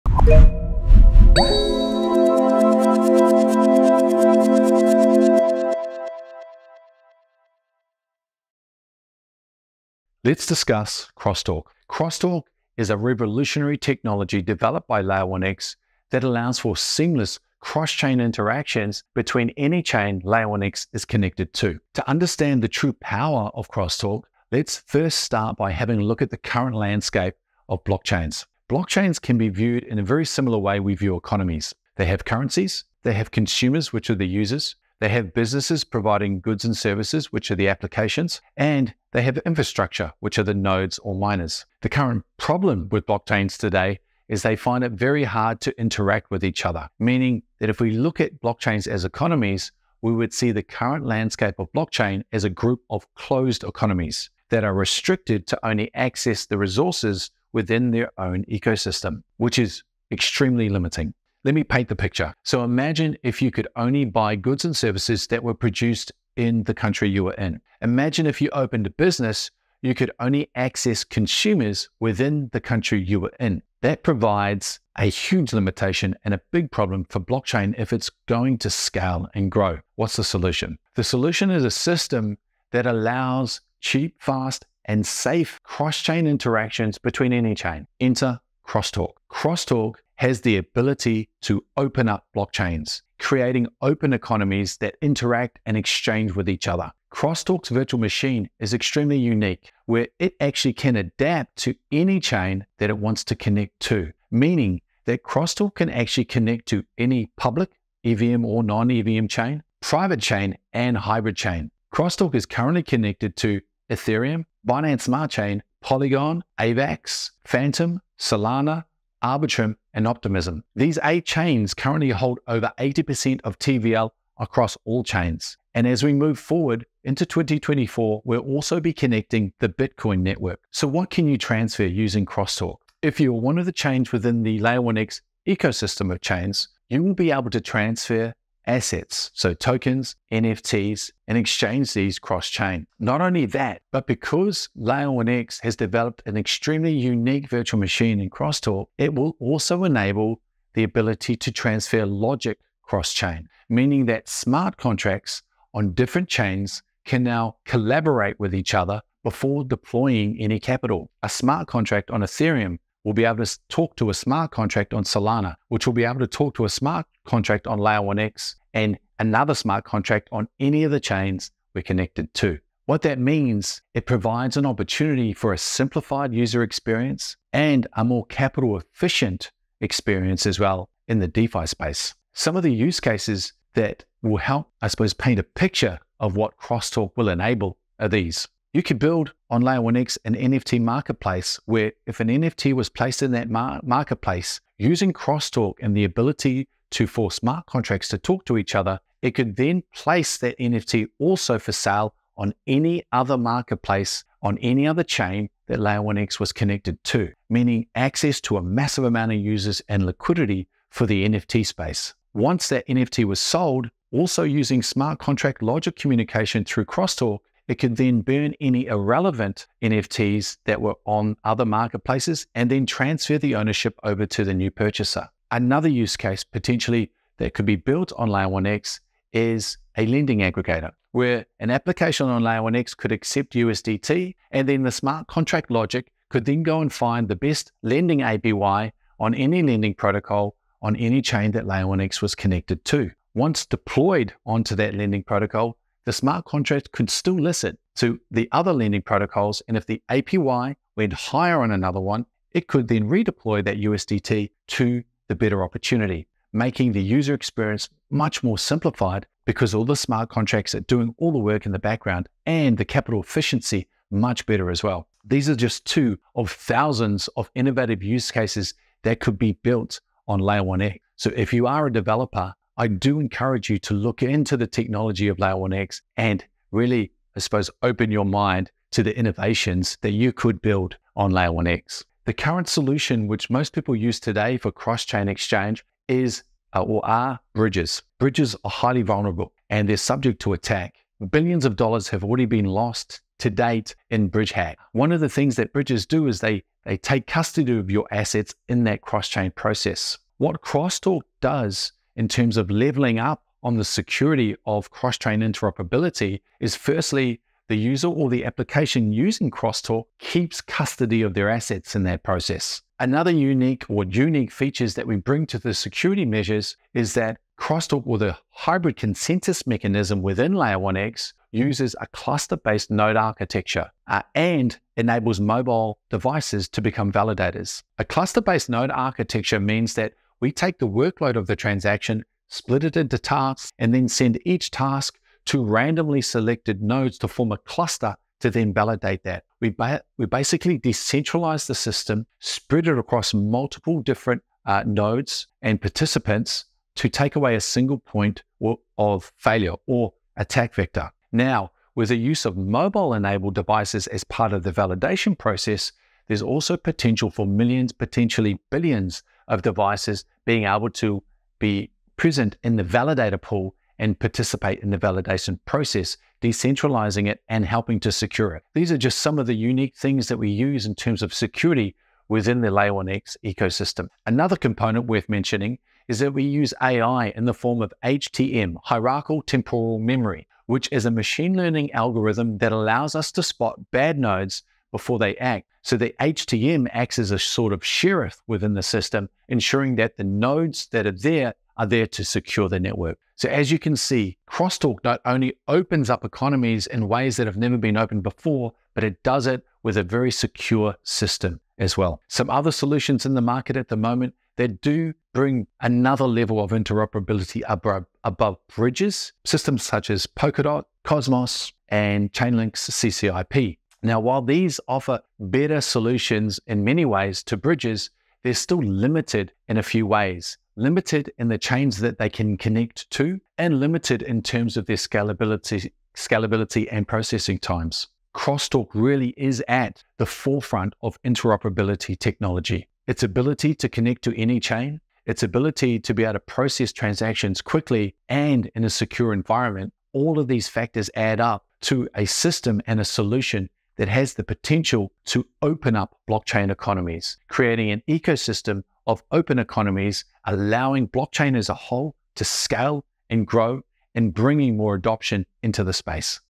What is X-Talk? The Future of Cross-Chain Interactions with Layer One X
Welcome to the world of 'X-Talk'!

In this video, Matiu Rudolph, COO of Layer One X, delves deep into the essence of crosstalk, a pioneering technology that promises seamless cross-chain interactions.

Understand the challenges of current blockchain landscapes and how crosstalk aims to revolutionize it. From the intricacies of closed economies in blockchain to the vulnerabilities of bridges, discover how Layer One X's unique approach to security, scalability, and interoperability is setting new standards. Dive into real-world use cases, from NFT marketplaces to lending aggregators, and envision the limitless possibilities.

If you're keen on blockchain innovations, this is a must-watch!